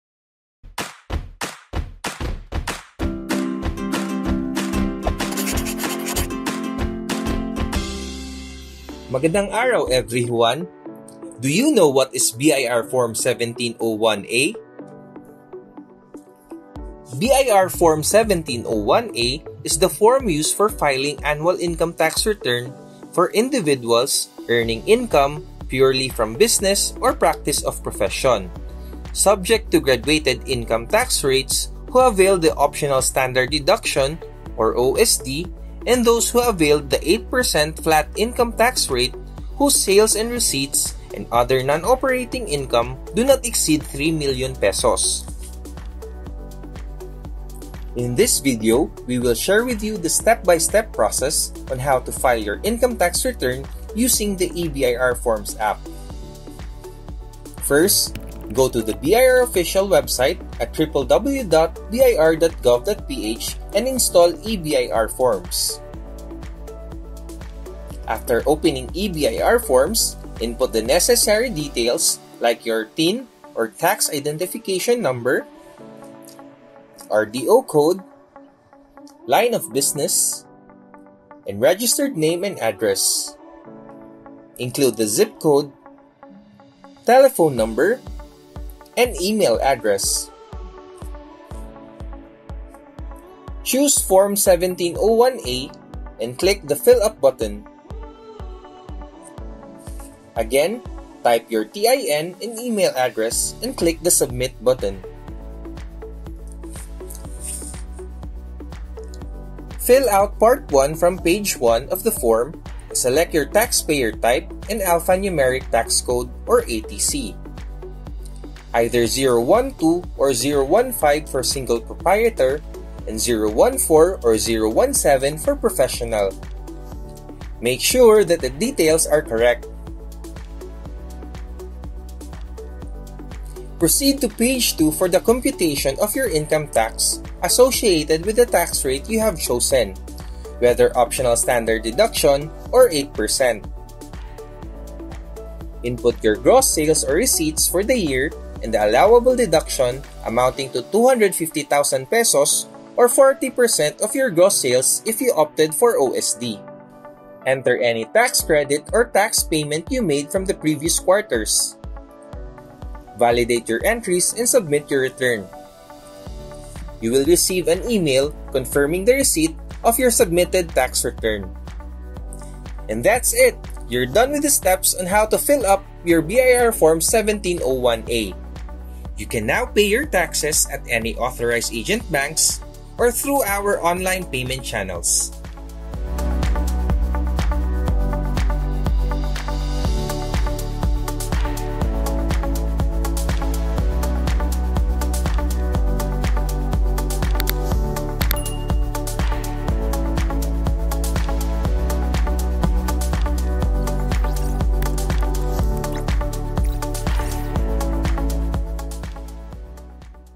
Step-by-step Guide in Filling Form 1701A using eBIR Forms
You may now file your annual income tax returns even without physically going to the revenue district office.
Here is a Step-by-step video guide in Filling Form 1701A using eBIR Forms.